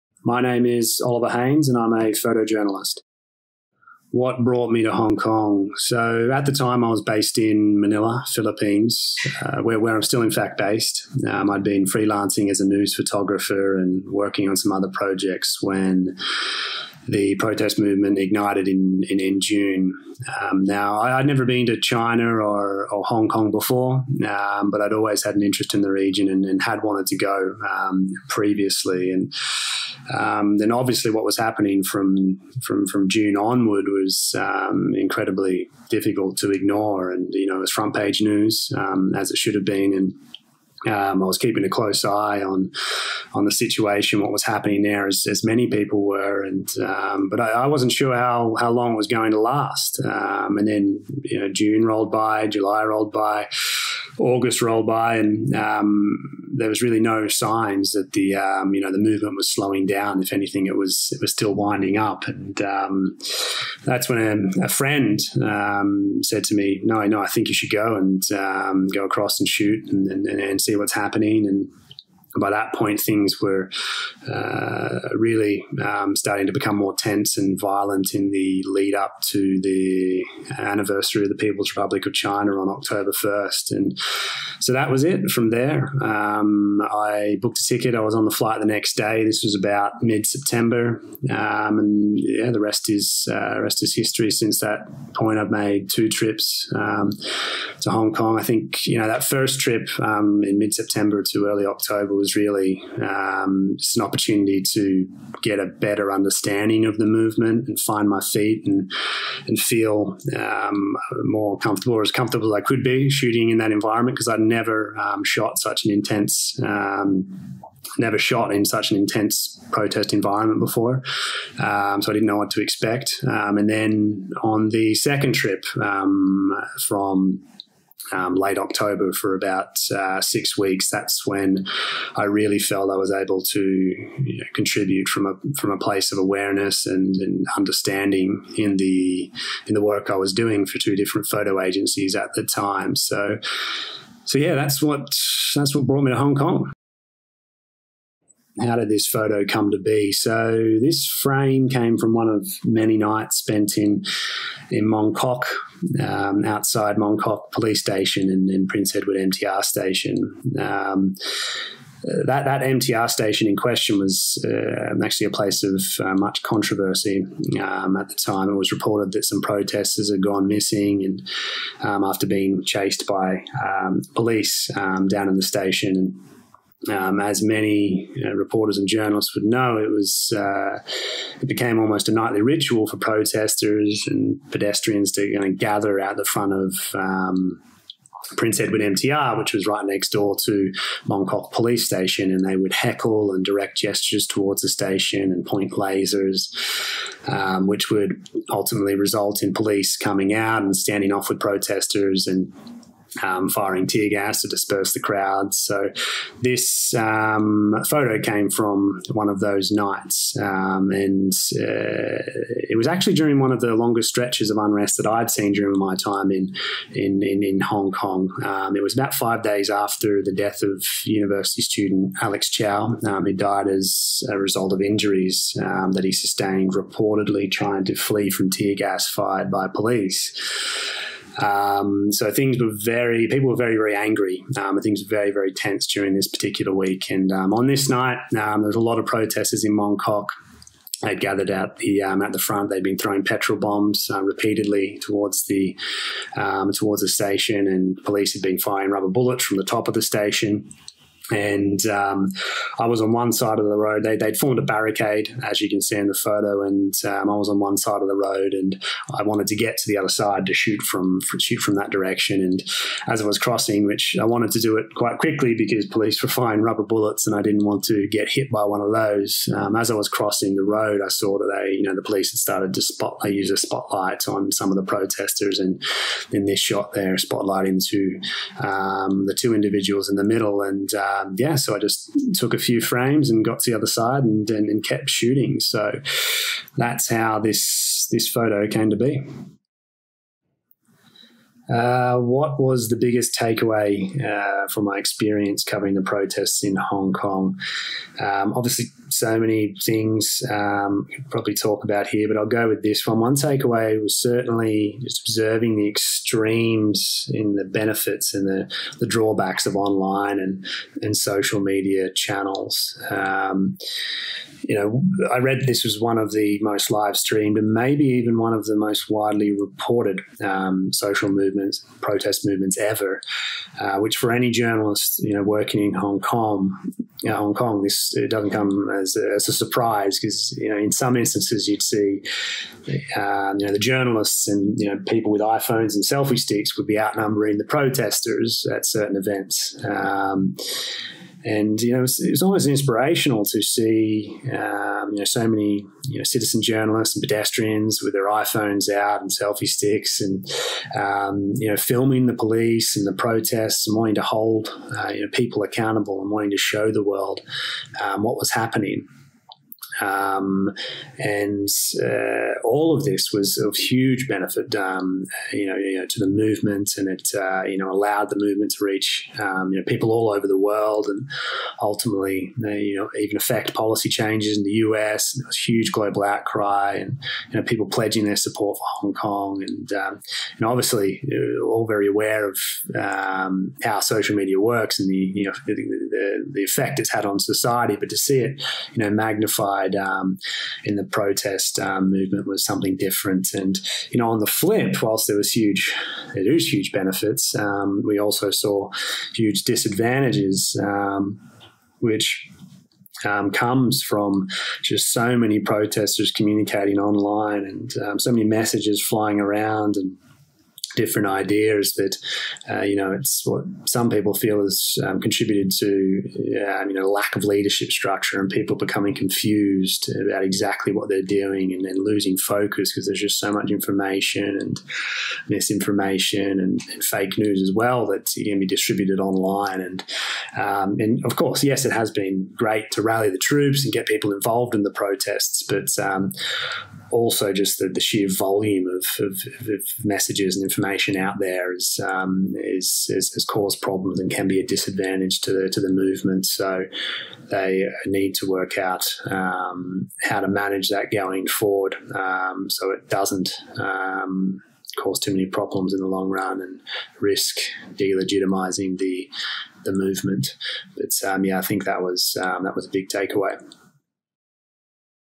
Hong Kong Protests: "Mong Kok Stand Off" By Oliver Haynes
Photojournalist Oliver Haynes talks about his photo, Mong Kok Stand-Off.

"Mong Kok: Protesters are illuminated by a police spotlight during a stand-off outside Mong Kok police station. Following the death of university student Alex Chow on November 8th, the financial hub was left reeling from one of the longest stretches of unrest since the pro-democracy movement began in June."

A collective of over 50 photojournalists who covered the 2019 Hong Kong protests.